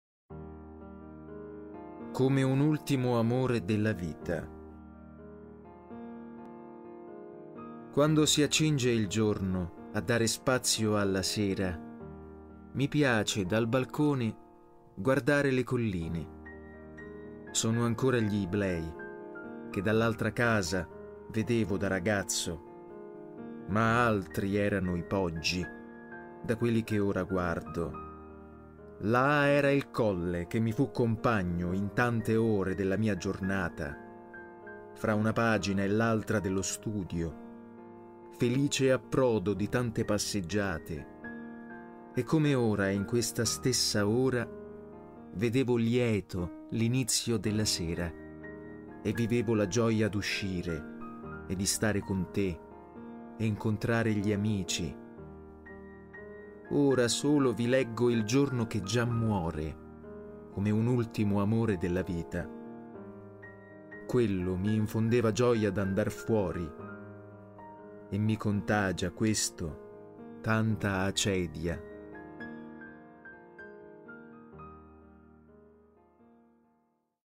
Benito Marziano 03 Come un ultimo amore della vita
Vive a Noto. Narratore e poeta. Scrive per Pagine, Roma, e altre case editrice. Con Ed. Urso ha pubblicato, Don Agostino Salvìa e altri racconti, II Crispiano 2008; II Mesagne 2013; I Custonaci 2013; Randagi, III Mesagne, 2013; Altri anni, IV Mesagne 2013; Sisifu, poesie; Juliette cara, romanzo. Varie selezioni.